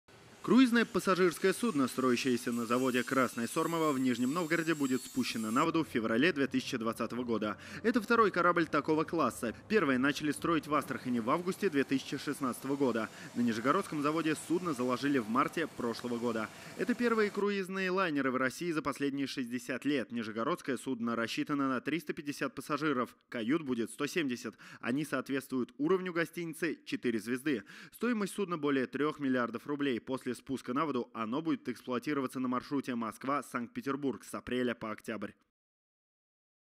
Круизное пассажирское судно в Нижнем Новгороде будет спущено на воду в феврале 2020 года
Это второй корабль такого класса. Первый начали строить в Астрахани в августе 2016 года. На нижегородском заводе судно заложили марте прошлого года. Это первые круизные лайнеры в России за последние 60 лет.